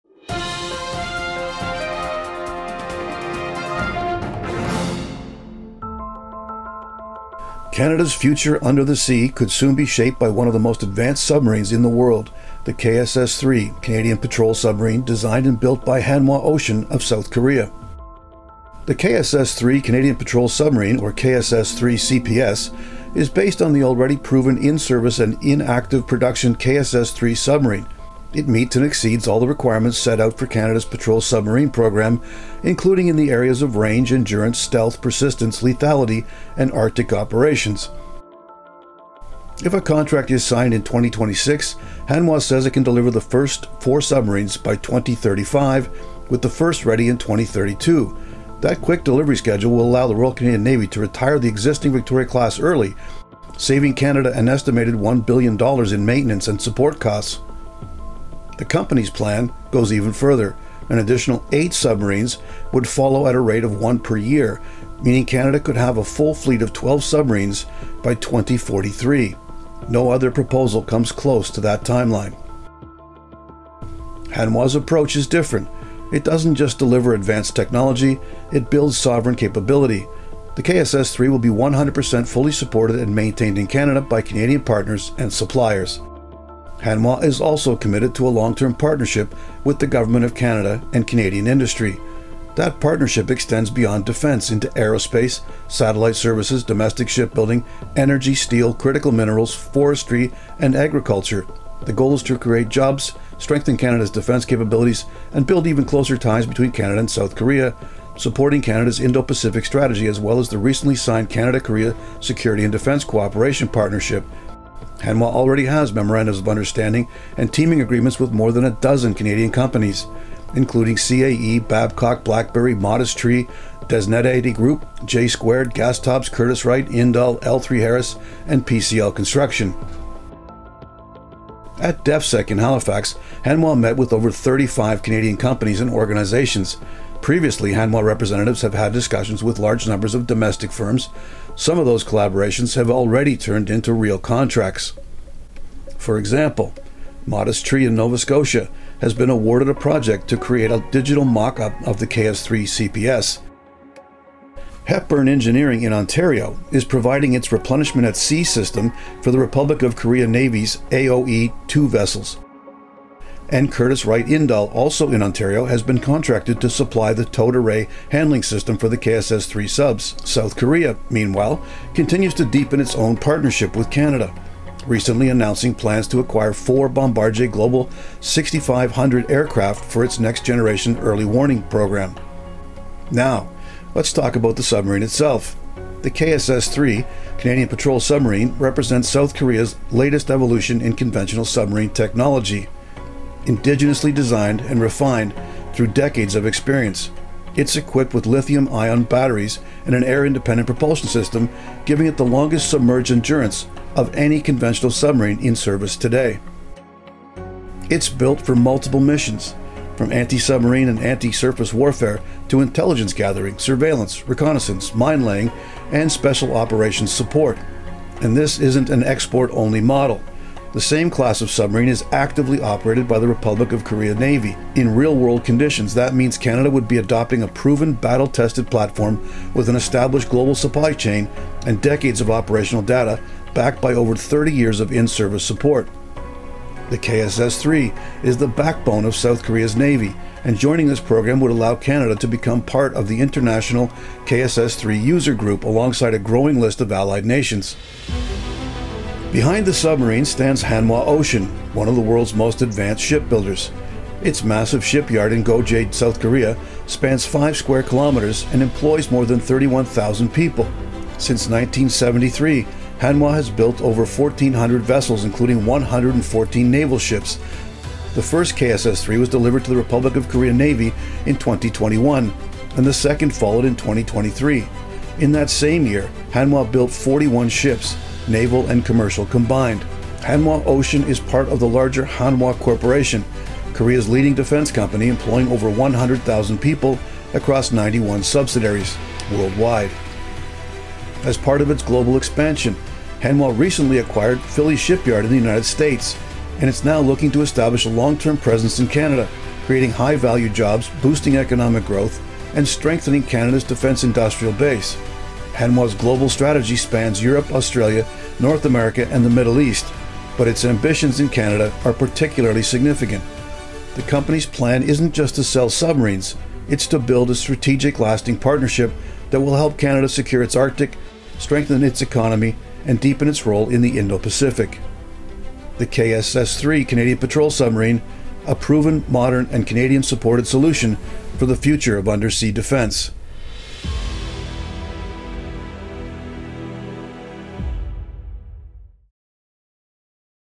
THE FUTURE OF THE ROYAL CANADIAN NAVY: Korea's KSS-III Submarine
🇨🇦 Canada’s Next Submarine: The KSS-III Canadian Patrol Submarine | Hanwha Ocean 🇰🇷
Canada’s future under the sea could soon be defined by the KSS-III Canadian Patrol Submarine (CPS) — one of the most advanced conventional submarines ever built, designed by Hanwha Ocean of South Korea.

Built for Arctic operations, stealth, and endurance, the KSS-III CPS meets and exceeds Canada’s Patrol Submarine Program requirements. With lithium-ion batteries and Air Independent Propulsion, it offers unmatched submerged endurance and advanced capabilities for the Royal Canadian Navy.

If approved by 2026, Hanwha can deliver the first subs by 2032, saving Canada over $1 billion and supporting Canadian jobs through full domestic maintenance and supply partnerships.

This isn’t just a submarine — it’s a strategic partnership between Canada and South Korea, strengthening defence, shipbuilding, aerospace, and energy collaboration.